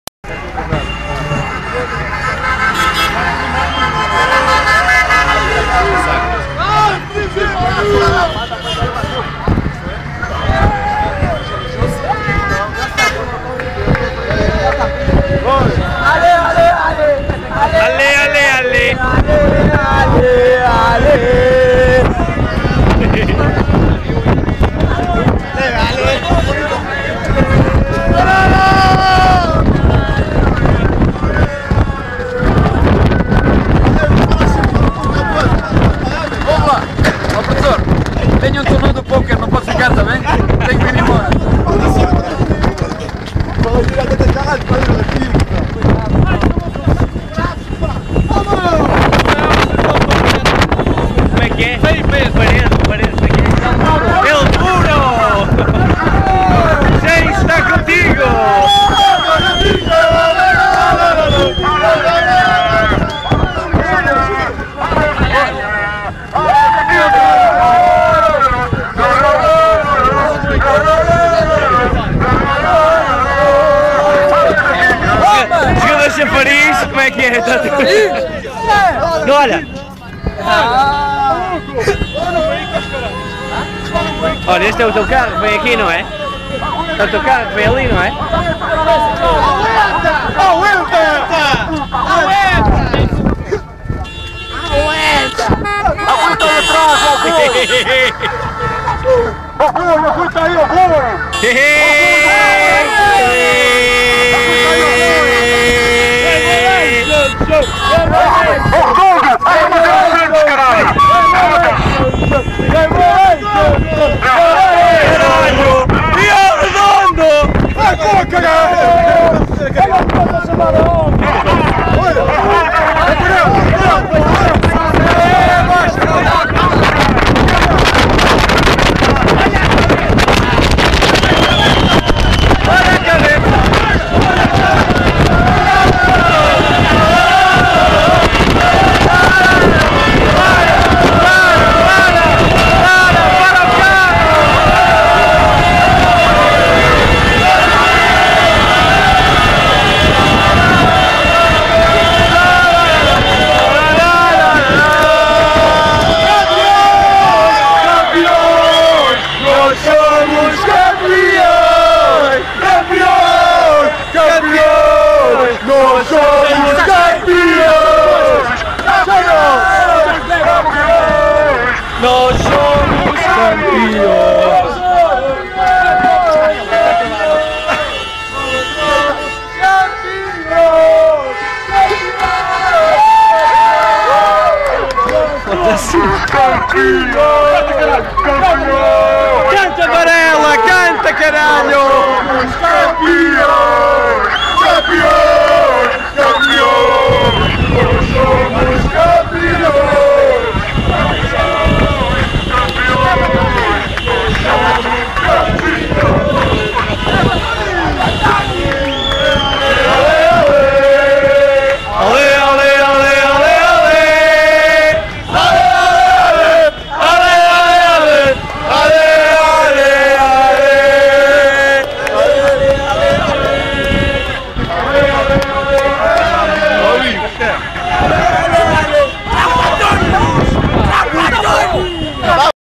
Beira mar Festa Subida 2010
Festa de subida á primeira liga do Beira-Mar na época de 2009/2010. Video do trajecto efectuado entre o estádio e o centro de Aveiro!!!